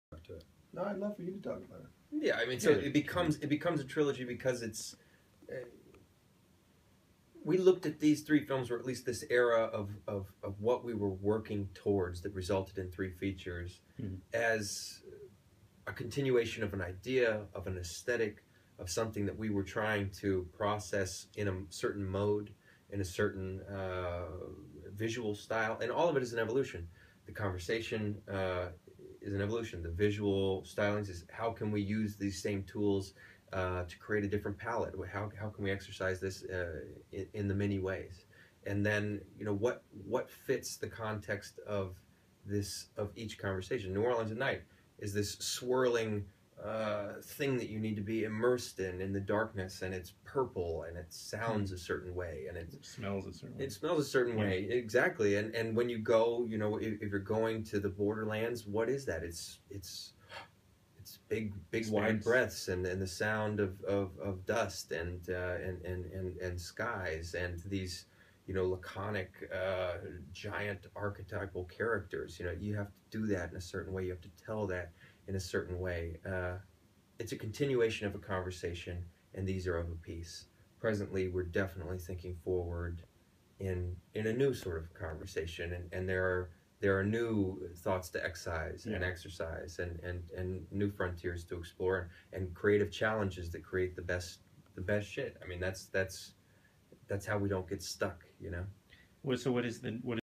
The Ross Brothers discuss WESTERN
The Ross Brothers, Bill (left) & Turner, discuss their latest documentary, WESTERN, which is the 3rd in a trilogy of films that might be referred as Americana themed. WESTERN recently had its NY premiere at the New Directors New Films series where this podcast interview was recorded. This is in the green room at the Elinor Bunin Munroe Film Center at Lincoln Center. 3/23/2015.